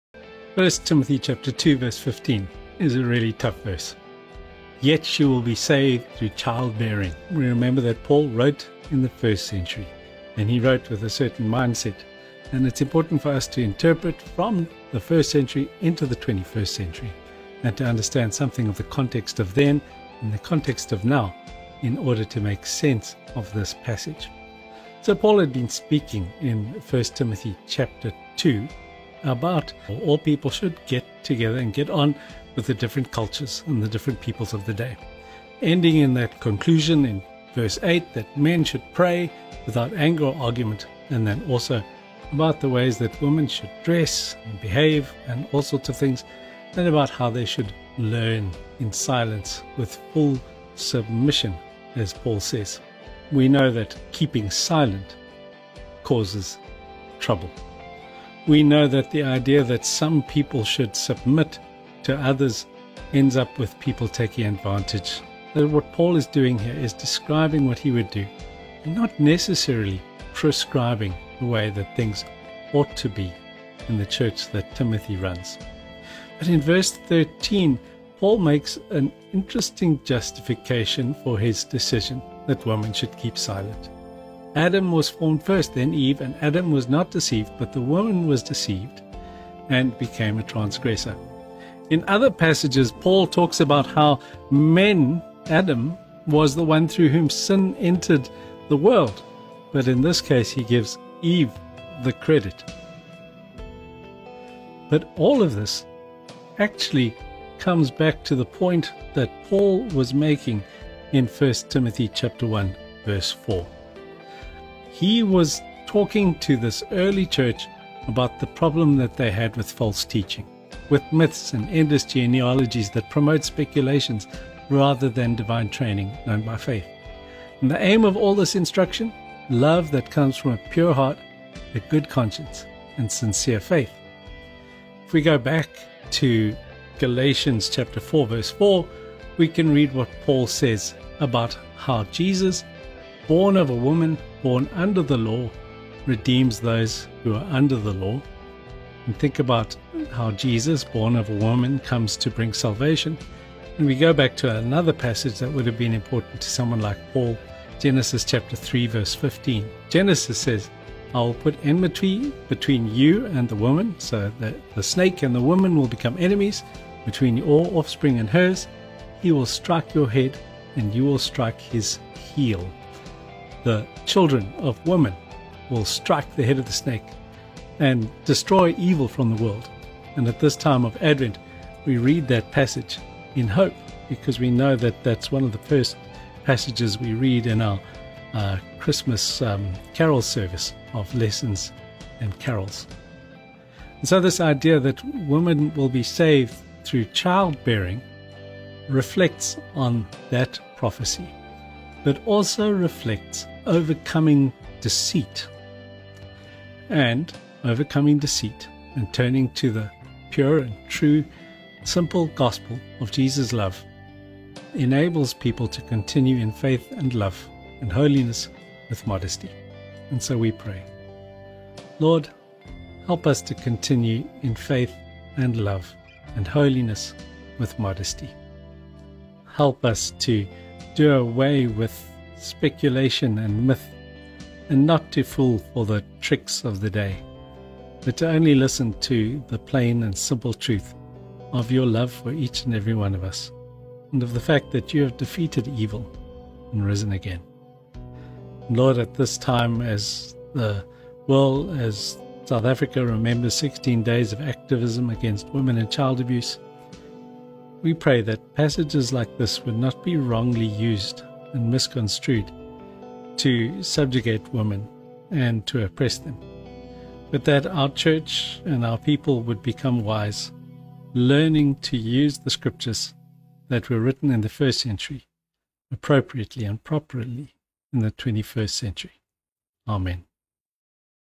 1 Timothy 2:13-15
I disagree with the idea that woman should be 'silent' or 'not have authority over a man' (1 Tim 2:11-12). I think these verses need to be carefully and properly interpreted.

But the tough one is verse 15 - "Yet she will be saved through child bearing..." or verse 14 that blames Eve for all the sin in the world.

I think it is important to consider the 1st century and the 21st century when you read and interpret. And to take responsibility for one's interpretation of scriptures. Some scriptures are 'descriptive' more than 'prescriptive'.

So here is an understanding. I'm sure its not the only one - and quite sure its not the best. But something to think about...

1 Timothy 2:13-15 For Adam was formed first, then Eve; 14 and Adam was not deceived, but the woman was deceived and became a transgressor. 15 Yet she will be saved through childbearing, provided they continue in faith and love and holiness, with modesty.